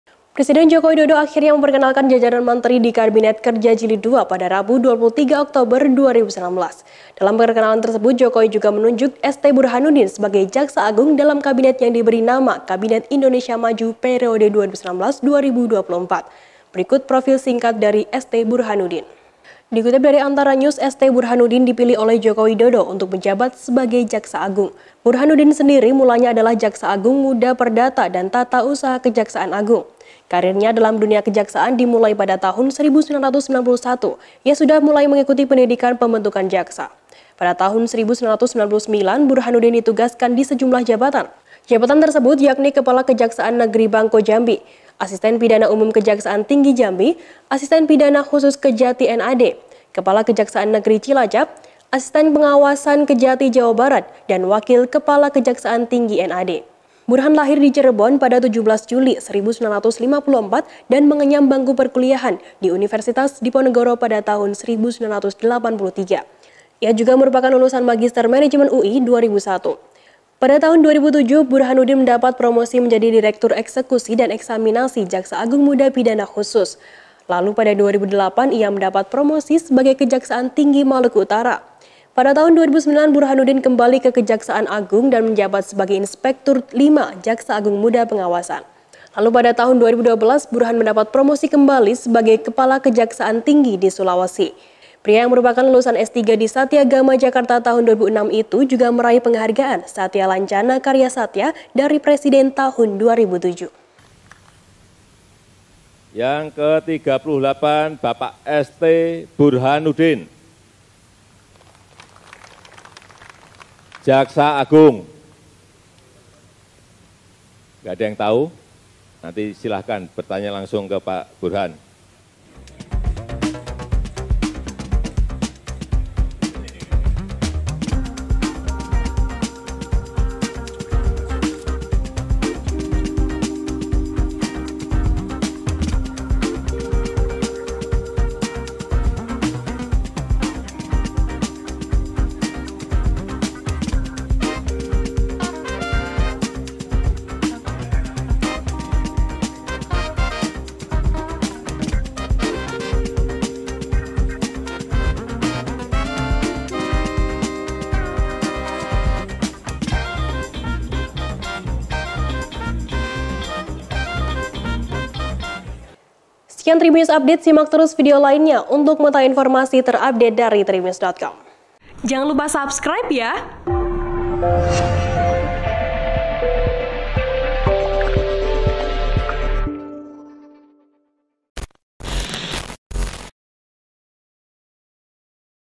ST Burhanuddin Ditunjuk Jadi Jaksa Agung, Berikut Rekam Jejaknya
TRIBUN-VIDEO.COM - Presiden Joko Widodo akhirnya memperkenalkan jajaran menteri yang akan dnuduki kursi di Kabinet Kerja Jilid II, Rabu (23/10/2019).

Dalam perkenalan tersebut, Jokowi juga menunjuk Burhanuddin sebagai Jaksa Agung dalam Kabinet yang diberi nama Kabinet Indonesia Maju periode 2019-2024.

Berikut profil singkat dari Burhanuddin.

Dikutip dari Antaranews, ST Burhanuddin dipilih oleh Joko Widodo untuk menjabat sebagai Jaksa Agung.

Burhanuddin sendiri mulanya adalah Jaksa Agung Muda Perdata dan Tata Usaha Kejaksaan Agung.

Kariernya dalam dunia kejaksaan dimulai pada tahun 1991. Ia sudah memulai mengikuti Pendidikan Pembentukan Jaksa.

Pada tahun 1999 Burhanuddin ditugaskan di sejumlah jabatan. Jabatan tersebut yakni Kepala Kejaksaan Negeri Bangko Jambi, Asisten Pidana Umum Kejaksaan Tinggi Jambi, Asisten Pidana Khusus Kejati NAD, Kepala Kejaksaan Negeri Cilacap, Asisten Pengawasan Kejati Jawa Barat dan Wakil kepala Kejaksaan Tinggi NAD.

Burhan lahir di Cirebon, 17 Juli 1954 dan mengenyam bangku perkuliahan di Universitas Diponegoro tahun 1983.

Ia juga merupakan lulusan Magister Manajemen UI 2001.

Pada tahun 2007, Burhanuddin mendapat promosi menjadi Direktur Eksekusi dan Eksmainasi Jaksa Agung Muda Pidana Khusus.

Lalu pada 2008, ia mendapat promosi sebagai Kejaksaan Tinggi Maluku Utara.

Pada tahun 2009, Burhanuddin kembali ke Kejaksaan Agung dan menjabat sebagai Inspektur V Jaksa Agung Muda Pengawasan.

Lalu pada tahun 2012, Burhan mendapat promosi kembali sebagai Kepala Kejaksaan Tinggi di Sulawesi.

Pria yang merupakan lulusan S3 di Satyagama Jakarta tahun 2006 itu juga meraih penghargaan Satyalancana Karya Satya dari presiden tahun 2007.

(Tribun-Video.com/AntaraNews)